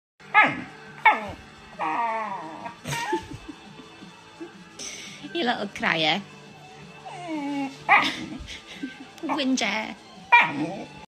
Cutest Chow Chow wants attention from his mommy
credits: @chycowchow